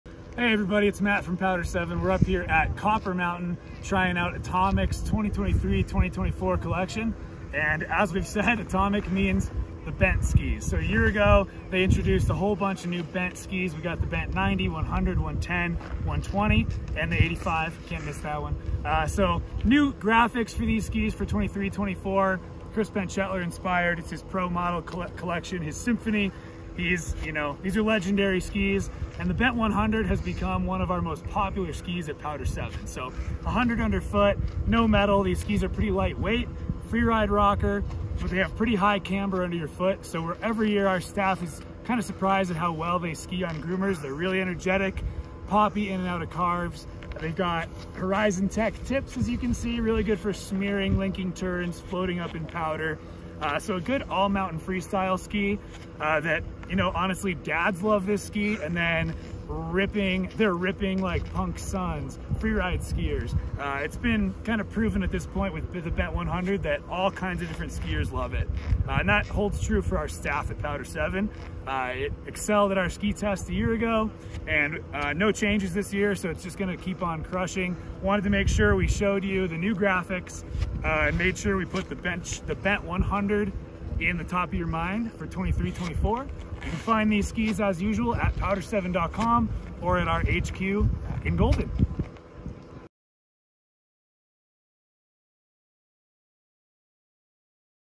2023-2024 Atomic Bent 100 First Look | Powder7
Since Atomic expanded the Bent Chetler collection into narrower waist widths, the Bent 100 has been a bestseller and customer favorite. Why? It blends all-mountain freestyle flavor with enough versatility and ease-of-use to meet the needs of a wide variety of skiers.

Here's our take:
When Atomic first released the Bent Chetler 100 skis a couple seasons ago, they thought the new all-mountain ski would appeal mostly to the young guns. But even they are pleasantly surprised with this ski’s widespread popularity. Sure, the punks love it. With twin tips, lots of pop, rad graphics, and smeary tips, the Bent 100 is built to turn the mountain into a playground. But it also more than holds its own in various terrain and snow conditions across the whole mountain. For one, it holds reliable edges at speed on groomers and carves with an addicting and snappy energy. It’s maneuverable enough to crush bumps yet wide enough for most realistic resort days (not to mention light enough to tour on). If directional skiing and all-mountain carving is your main hustle, you might check out the burlier Atomic Maverick 100 Ti instead. But if you like to ski loose and free on some runs and then drop the hammer on others, the Bent 100 is the boat to take you there.